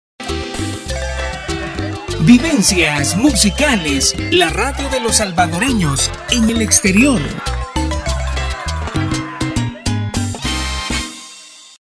RADIO DE LOS SALVADOREÑOS EN EL EXTERIOR AL AIRE
Somos una radio on line con una programación variada, con espacios de difusión de música salvadoreña, información y mensajes positivos. Transmitimos las 24 horas desde San Salvador, El Salvador